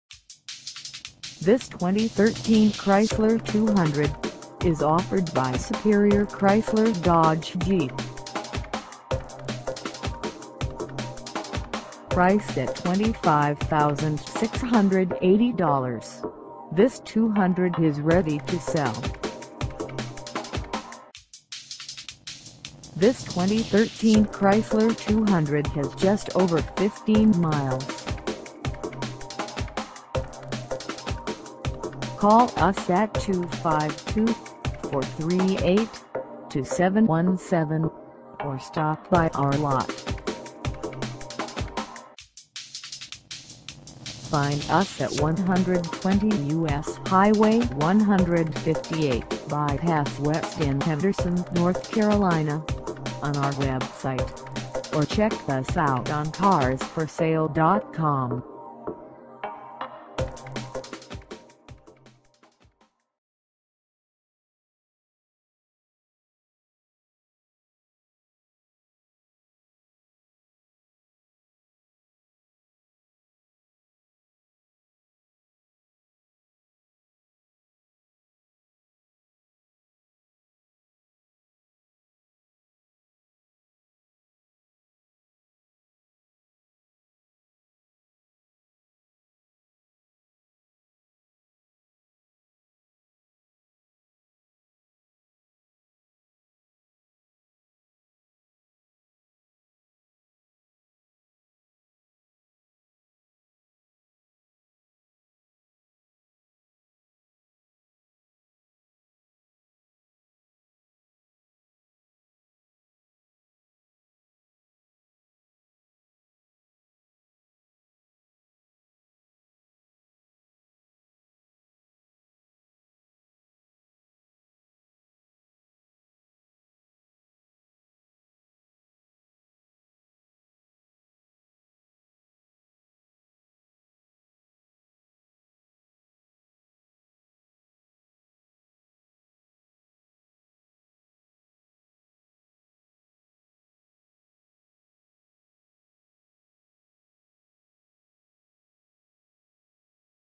2013 Chrysler 200 Limited - for sale in HENDERSON, NC 27536
SUPERIOR CHRYSLER DODGE JEEP
120 U.S. Highway 158 Bypass West

Come test dirve this 2013 Chrysler 200 Limited for sale in HENDERSON, NC.

Prepare to feel the sensation of this 2013 Chrysler 200's 3.6L V-6 cyl engine as you settle into the driver's seat. Finished in attractive Bright White, this Sedan stands out in its class. Call today and we'll tell you more about this exceptional vehicle!